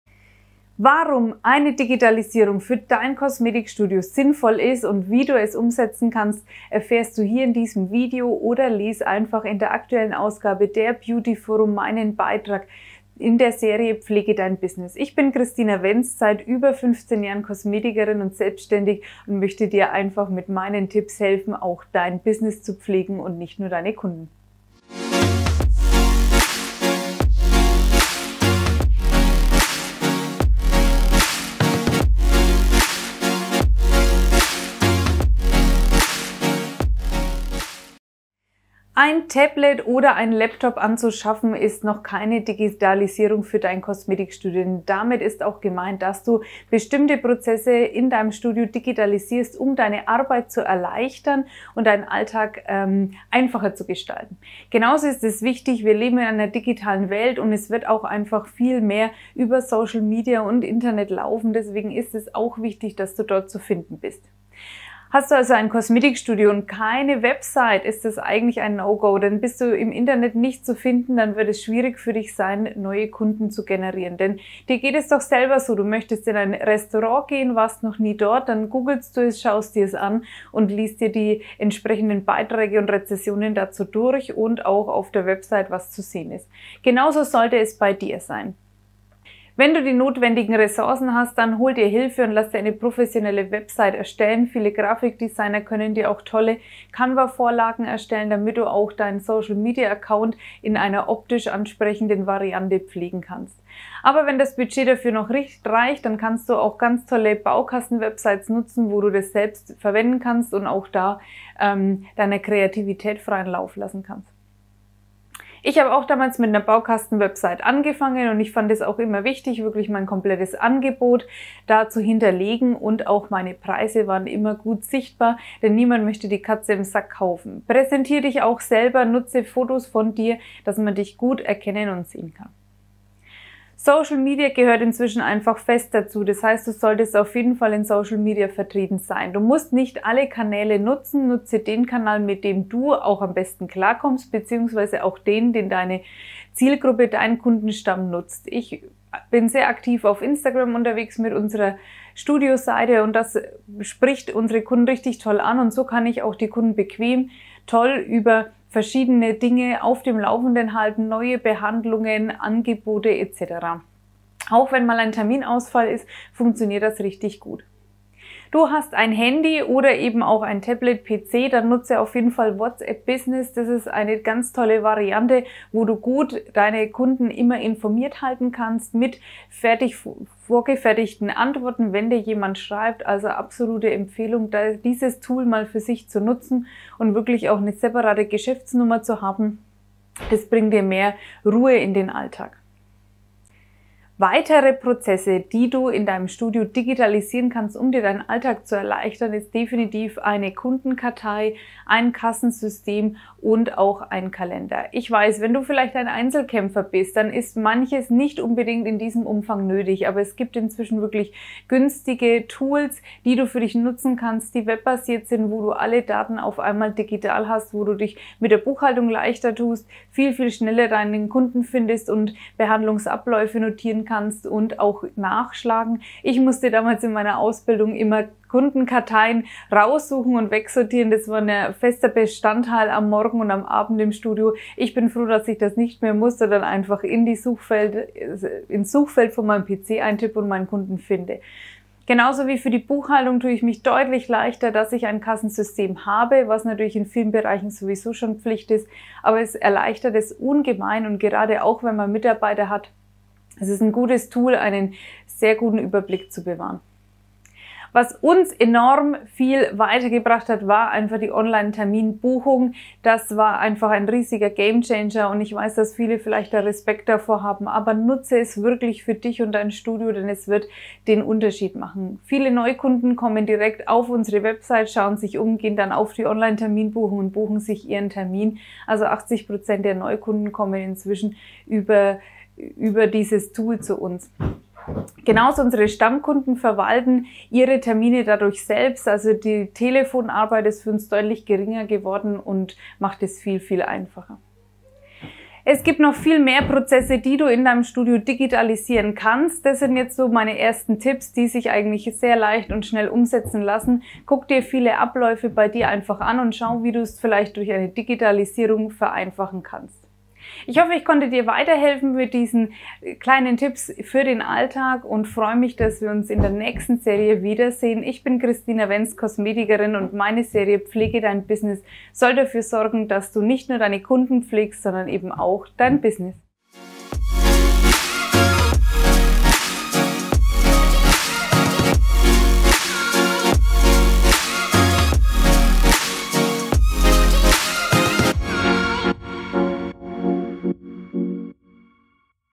Teil 9: So geht Digitalisierung im Kosmetikinstitut
Pflegen, pflegen, pflegen: Das ist das Credo für die Haut der Kundschaft und auch für das Business! Kosmetikerin und Beauty-Coach Christina Wenst gibt Tipps, wie das Kosmetikinstitut in Schuss bleibt. In Teil 9 geht es darum, Betriebsprozesse zu digitalisieren. Sie erfahren, warum sich eine
Umstrukturierung für Sie und Ihre Kunden lohnt und auf was Sie dabei achten müssen.